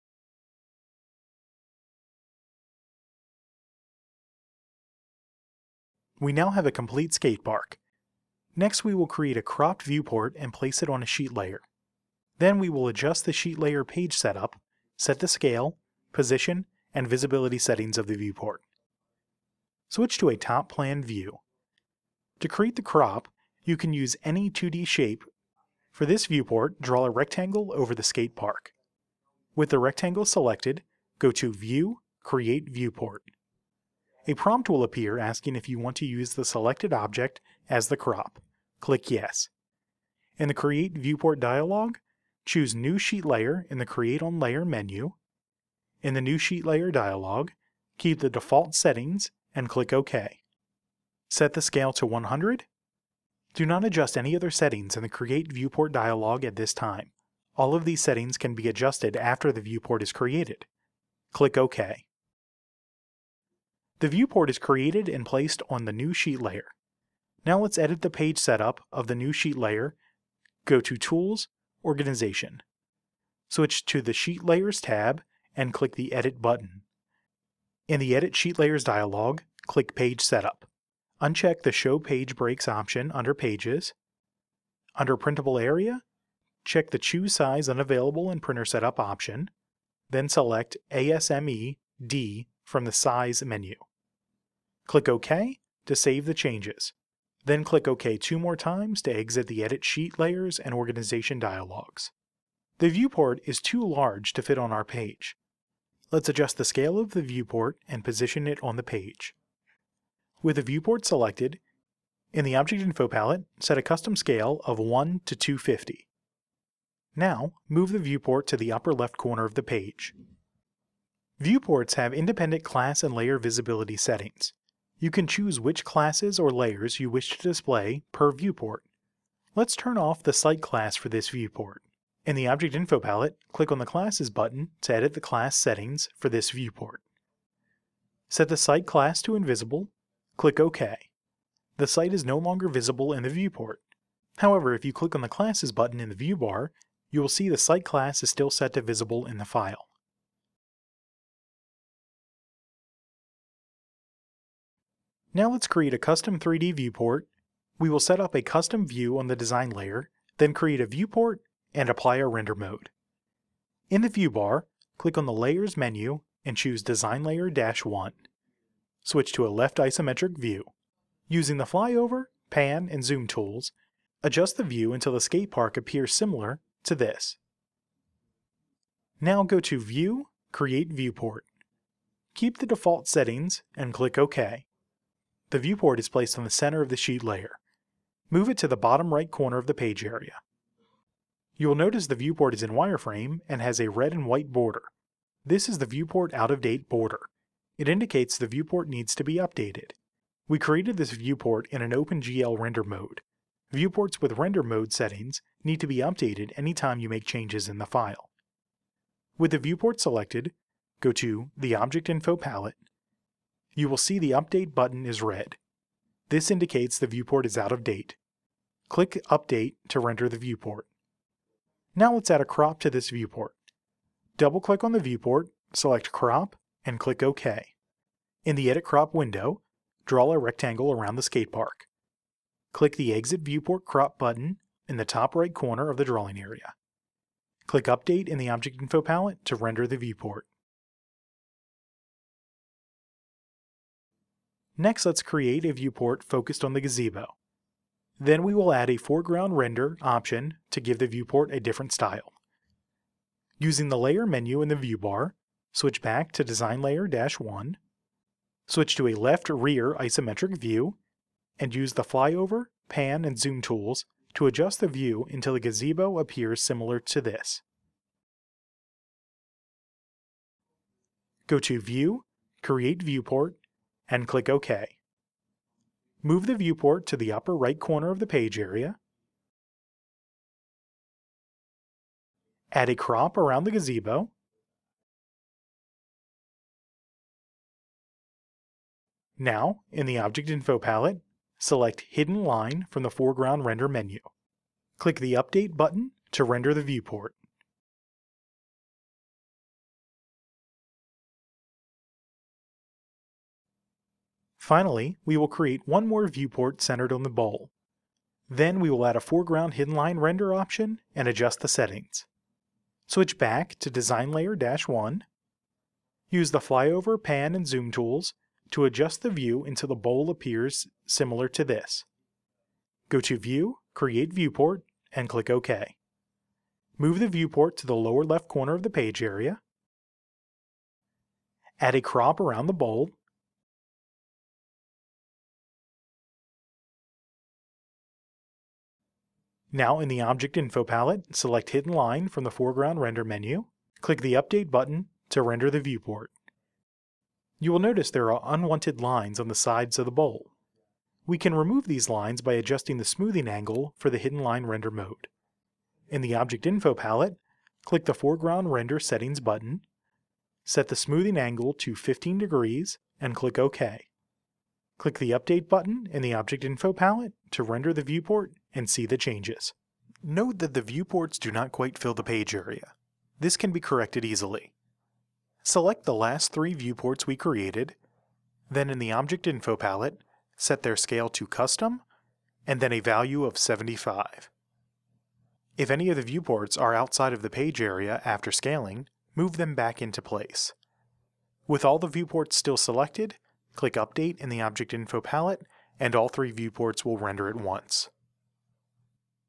GSG - Vectorworks Fundamentals 2016 - 12 Creating Viewports
Now that the Skatepark is finished, we will create a cropped viewport on a sheet layer. Then, we will adjust the sheet layer page setup, set the scale, position, and visibility settings of the viewport.